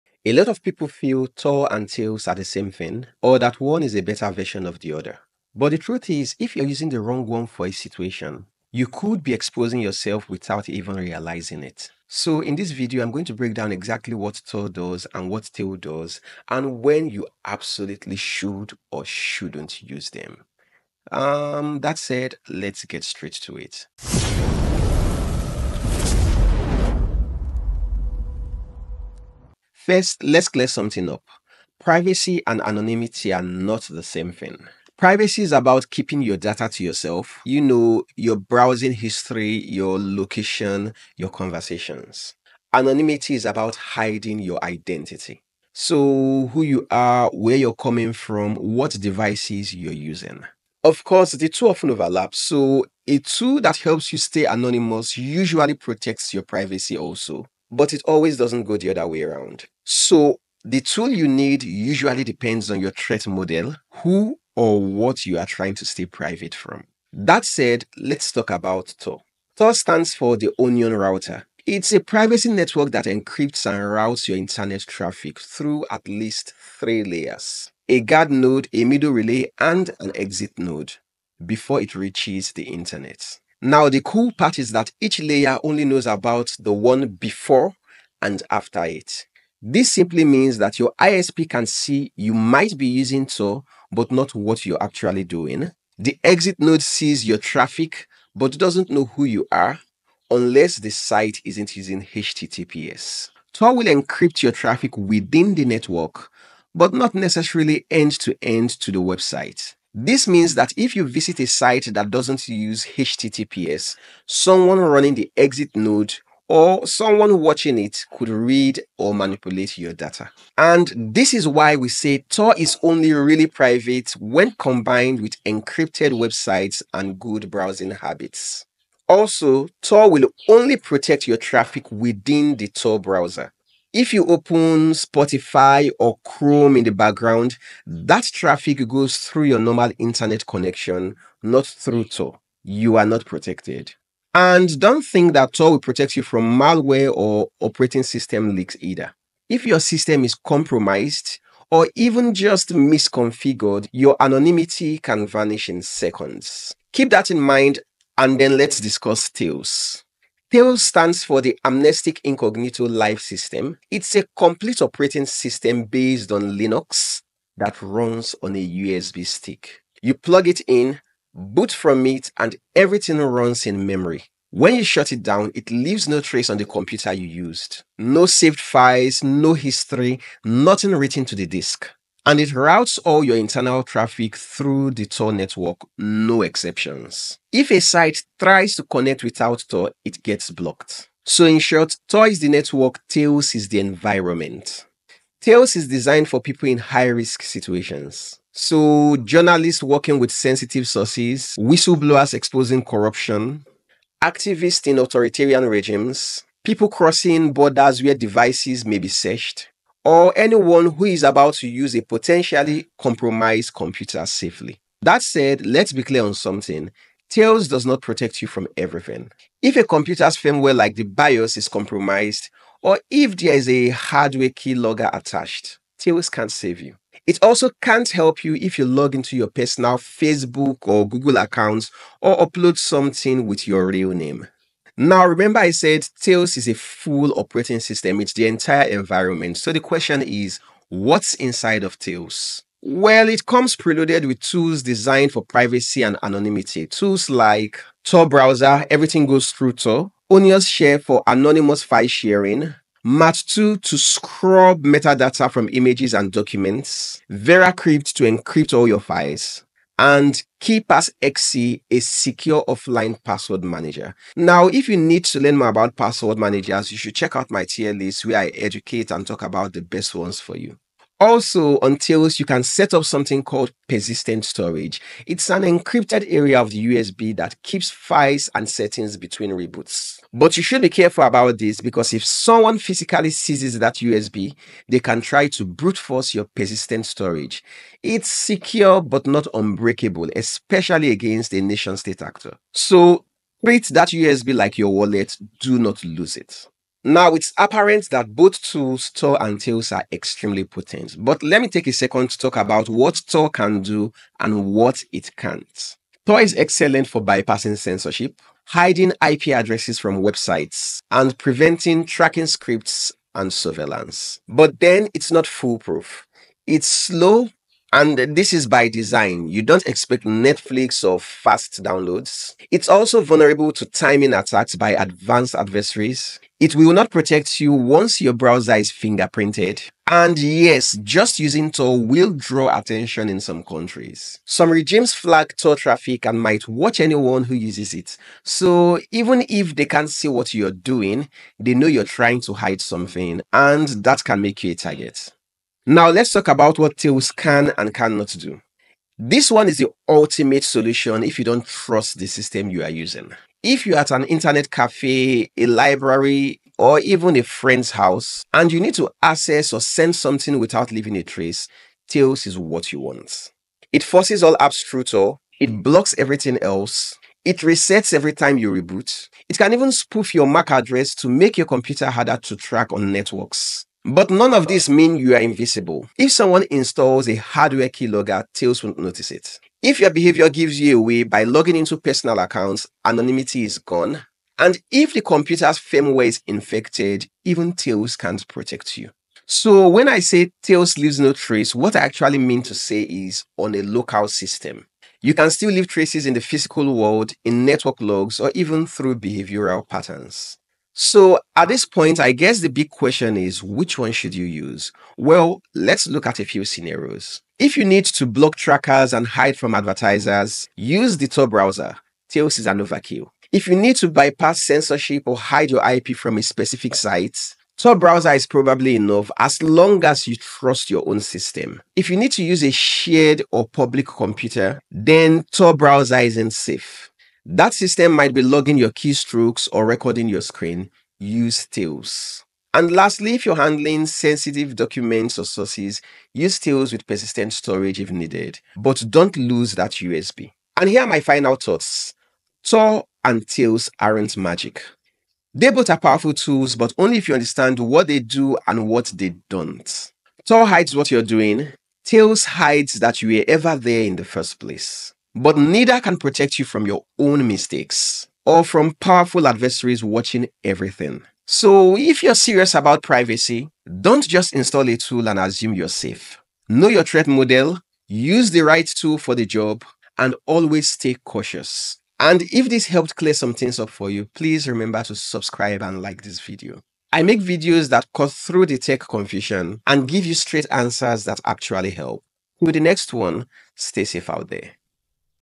Tor vs Tails: Use the Wrong One and You’re Exposed
Tor and Tails are NOT the same and using the wrong one could expose you.
In this video, I break down the critical differences between the Tor Browser and Tails OS, what each one actually protects you from, and when you should absolutely use one over the other.

I’ll walk you through how Tor works, what Tails is really for, common mistakes that could ruin your anonymity, and real-life scenarios to help you choose the right tool for your situation.

🔐 Topics Covered:

What Tor and Tails actually do (and what they don’t)

Privacy vs anonymity — the real difference

When Tor is enough, and when you need Tails

Real-world use cases and threat models

The tools built into Tails OS

Common risks and misconceptions

📌 Timestamps:

00:00 – Intro: Why This Matters More Than You Think
00:29 – Privacy vs Anonymity: What Are You Actually Protecting?
01:07 – What Is Tor and How Does It Work?
02:40 – What Is Tails OS and Why It’s Different
04:00 – What’s Inside Tails: Preloaded Tools and Features
04:59 – What Tor Can Do (and What It Can’t)
05:46 – What Tails Can Do (and What It Can’t)
06:40 – Which One Should You Use? Tor or Tails?